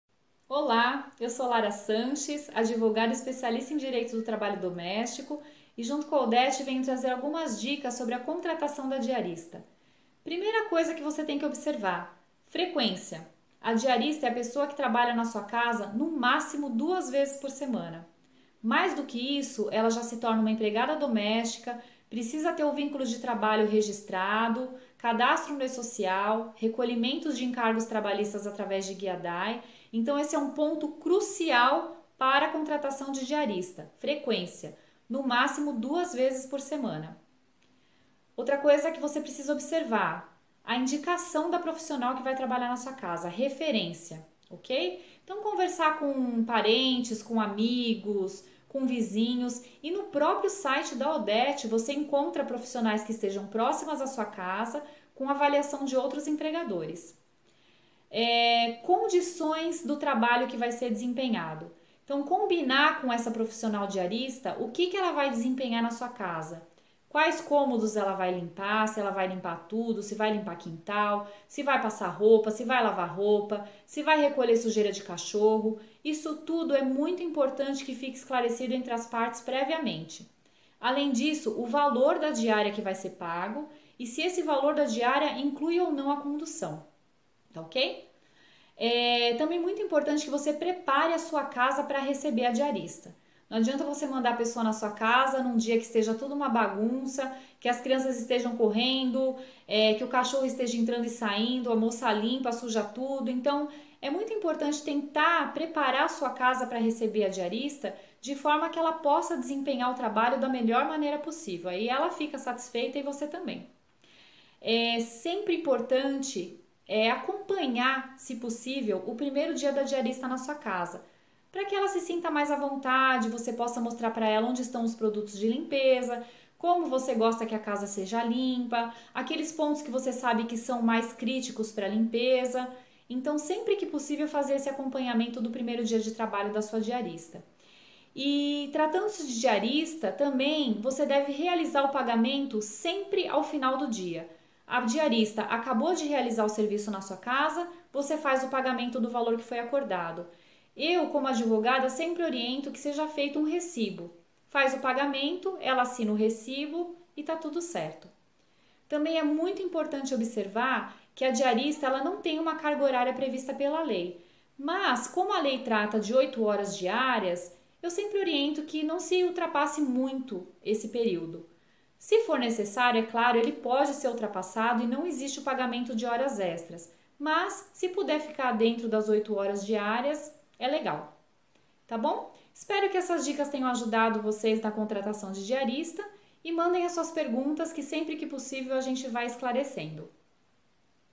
Dicas de contratação: Como contratar uma diarista?
Neste vídeo a Dra. Lara Sanches (Advogada especialista em direito do trabalho doméstico) nos trás dicas simples de como contratar uma diarista.

São dicas simples que servem tanto para a contratante como para a diarista.

1) Freqüência
Diarista só pode realizar o trabalho em sua casa, no máximo 2x na semana!

2) Indicação da profissional (referência)
Conversar com parentes, amigos, no próprio site da odete.com.br, temos referências de outros contratantes!

3) Condições do trabalho
Combinar o que ela vai desempenhar no dia de trabalho.

4) Valor da diária
Valor e se incluem ou não a condução e alimentação.

5) Preparar a casa
Não deixar crianças correndo, animais sujando a casa depois de limpa.

6) Acompanhar o primeiro dia
Ir mostrando os produtos, falar como gosta que a casa seja limpa, aquela cantinho que só você conhece.

7) Realizar o pagamento no dia!
Sempre ao final do dia e de preferência com um recibo!

8) Carga horário
Se possível, não deixe que sua diarista ultrapasse o limite de 8horas.

Espero que essas dicas sirvam para que diarista e contratantes tenham um casamento perfeito!